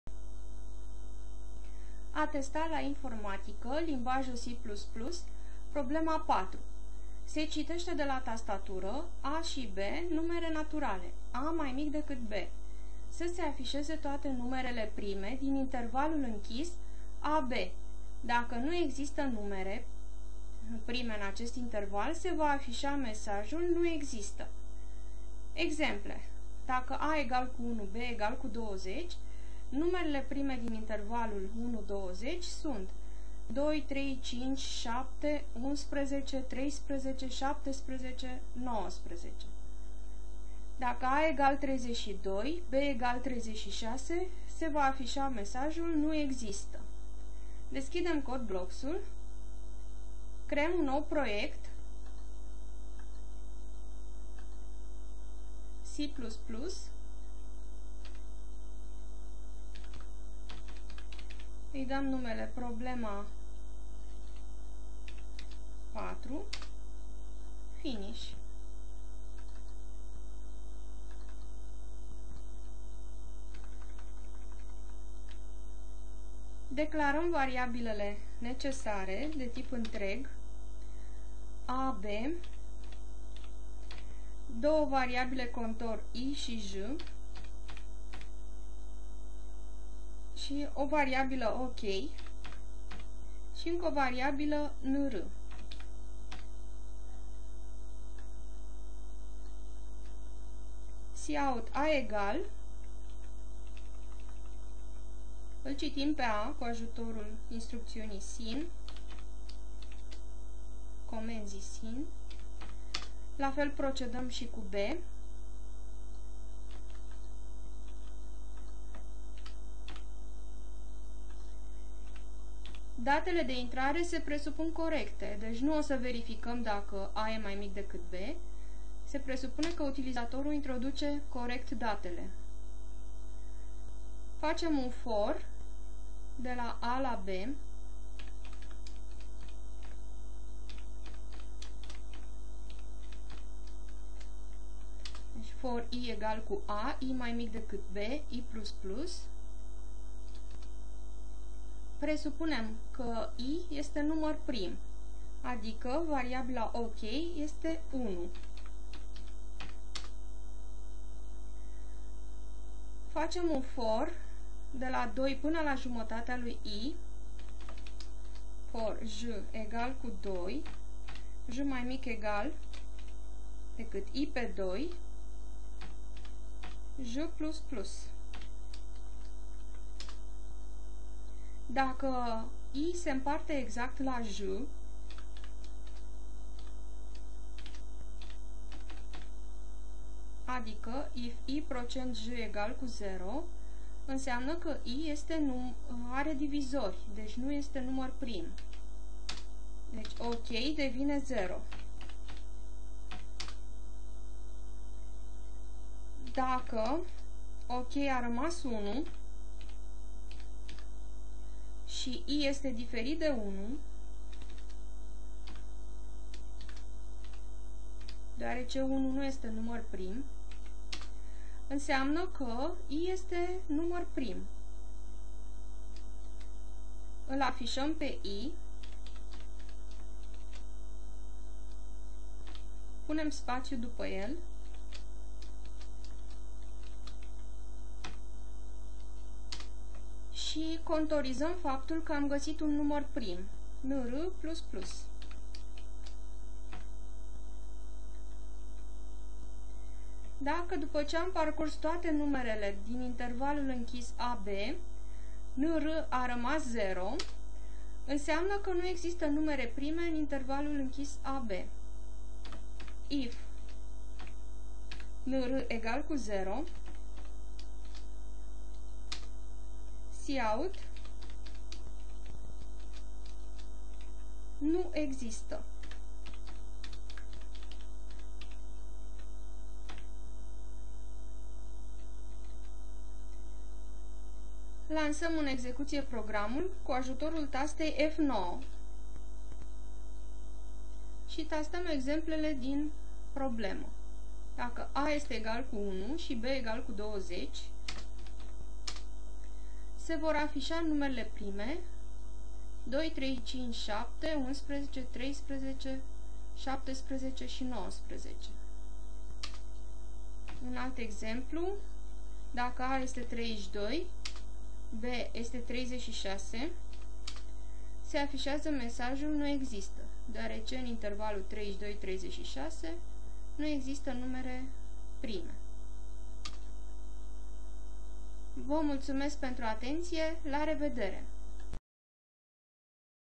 Limbajul C++ - problema rezolvata 4 - numere prime
Se citeşte de la tastatură a şi b numere naturale (a este mai mic decat b). Să se afişeze toate numerele prime din intervalul închis [a,b]. Dacă nu există numere, se va afişa mesajul NU EXISTA.